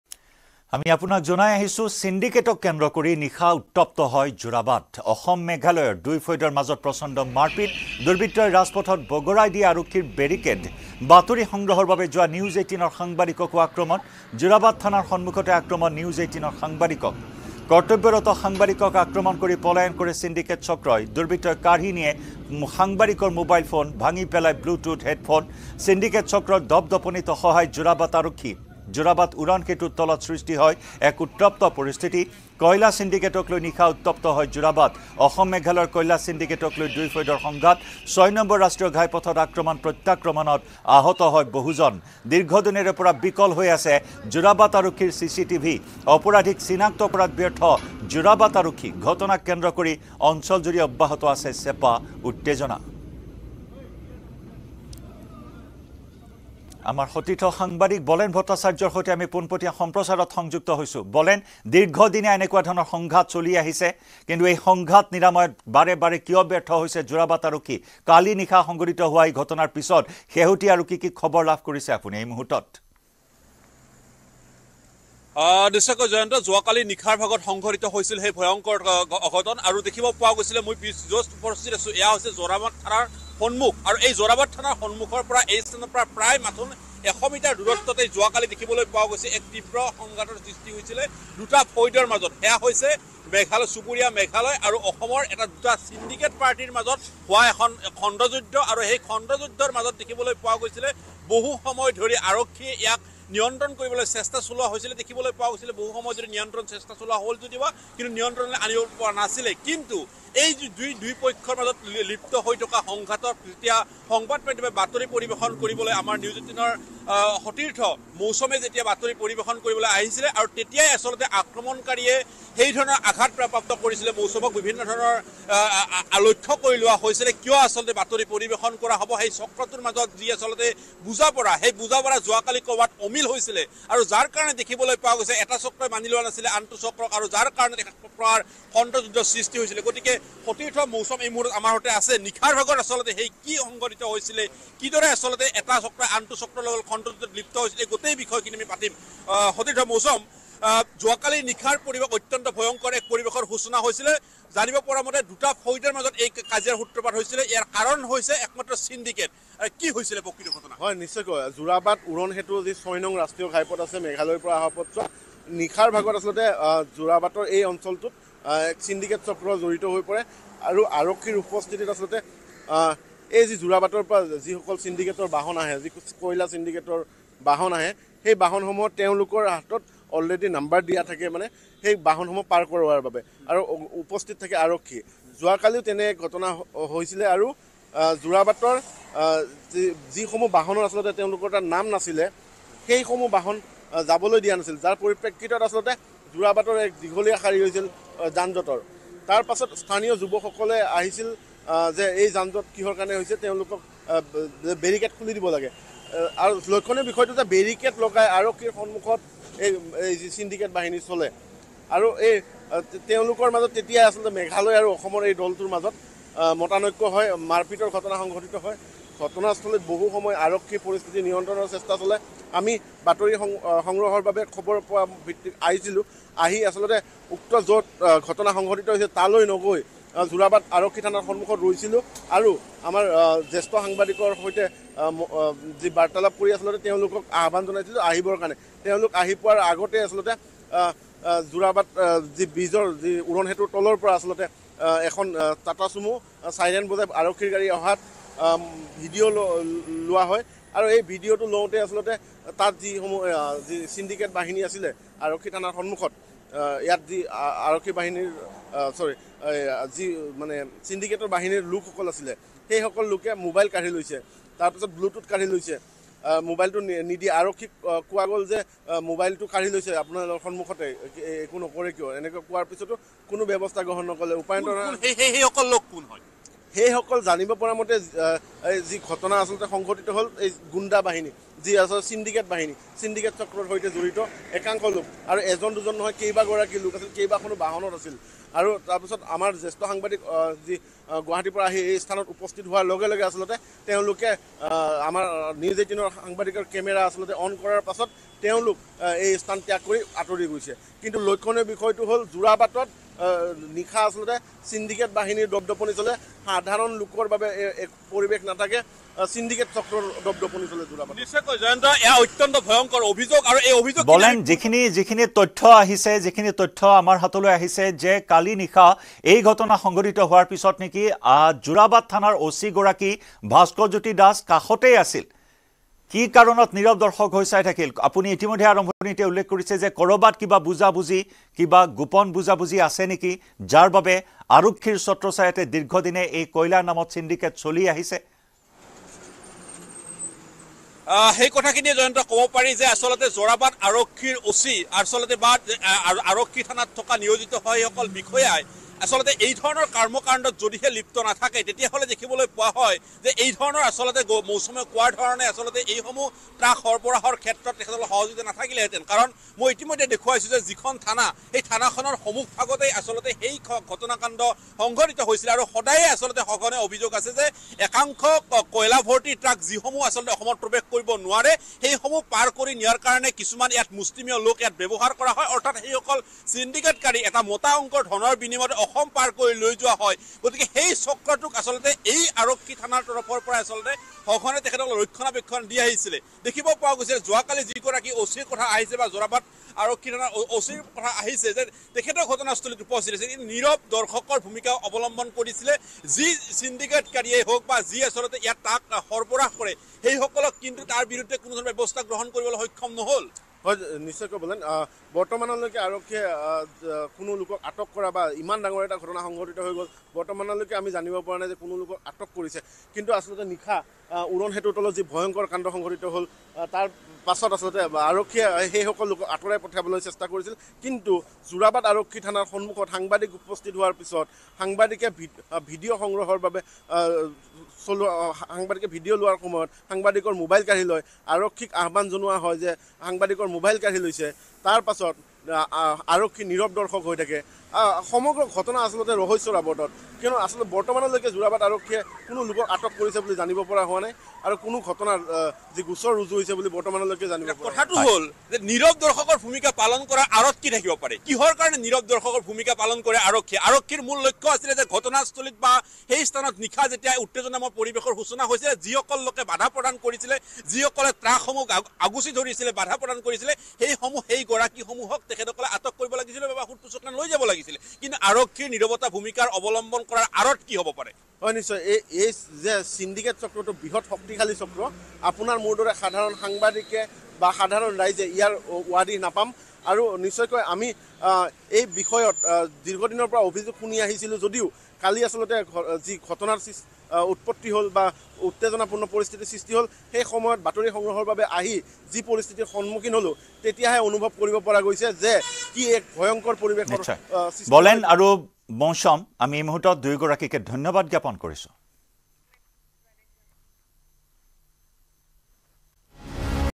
ছিণ্ডিকেটক লৈ Assam-Meghalaya আৰক্ষীৰ সংঘাত। বাতৰি সংগ্ৰহৰ বাবে যোৱা NEWS18ৰ সাংবাদিকক আক্ৰমণ।
লগতে চাওঁক দিনটোৰ বহু গুৰুত্বপূৰ্ণ খবৰ